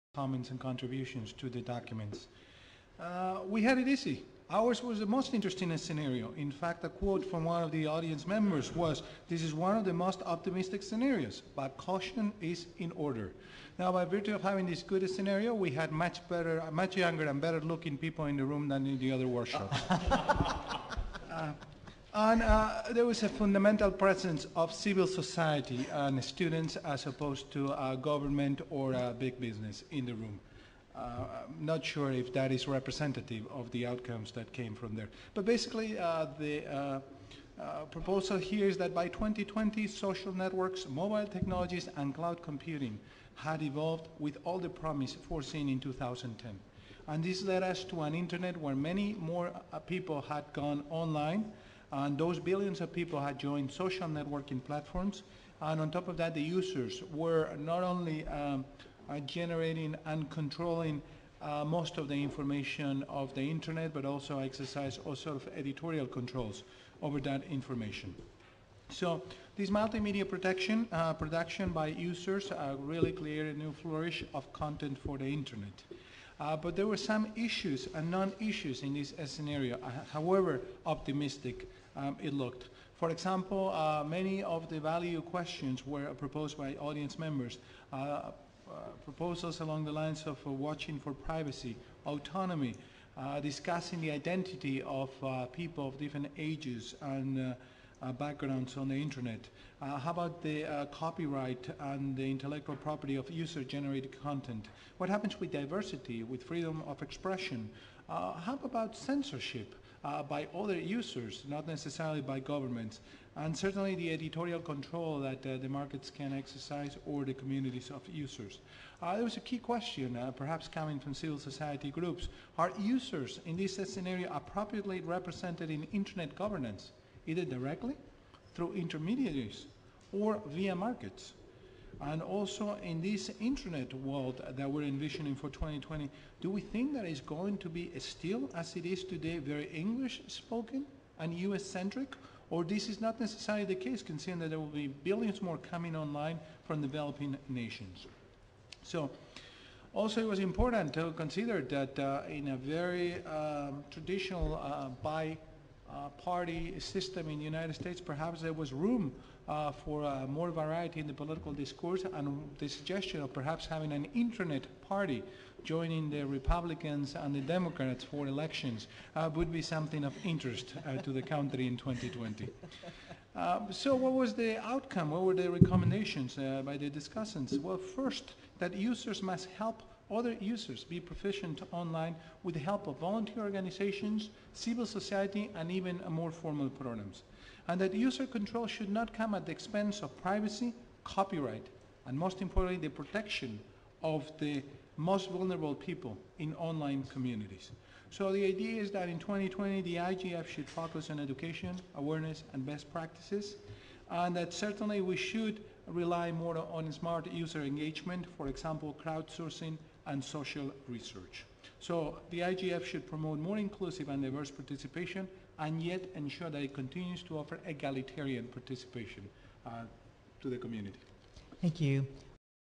IGF USA, 2010: Users reign report
Pablo Molina of Georgetown University shares primary points from the July futures-scenario discussion on the "Users Reign" scenario,which, he pointed out, has its own set of concerns.